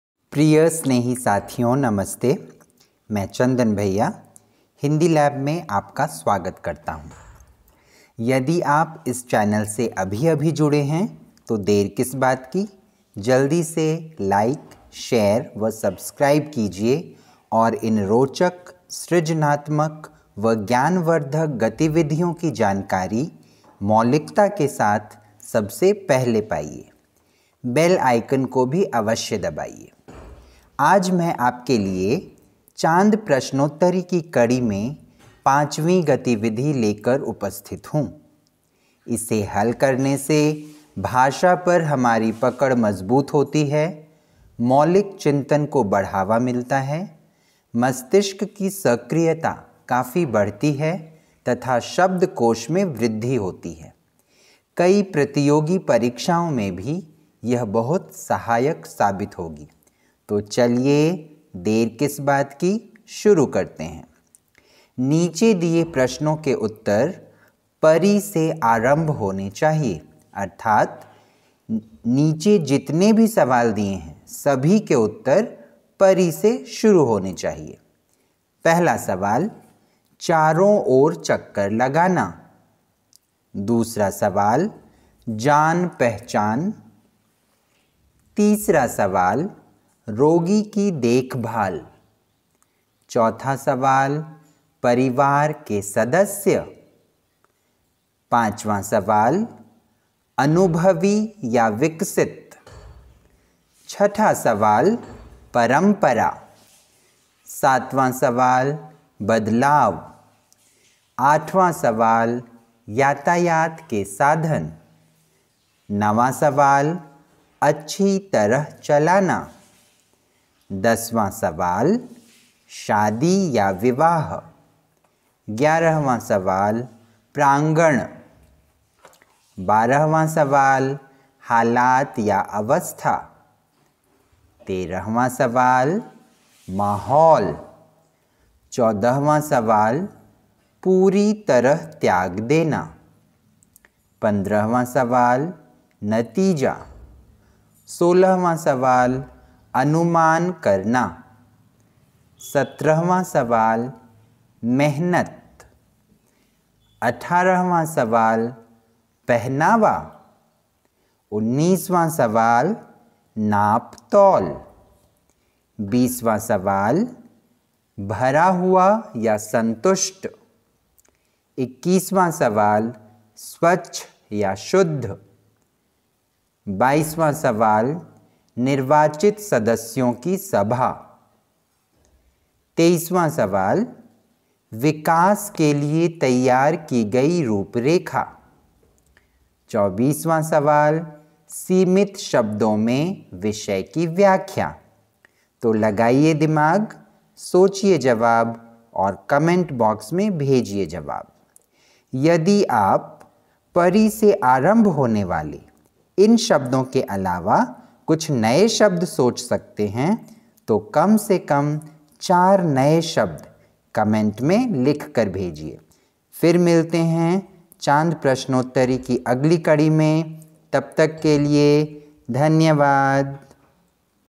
chand prashnottari 5 | hindi quiz 5 | interesting hindi activity | chandan singh 'chand' | hindi lab
आइए , दिमाग लड़ाएँ और 'चाँद' प्रश्नोत्तरी 5 को हल करें । सादर धन्यवाद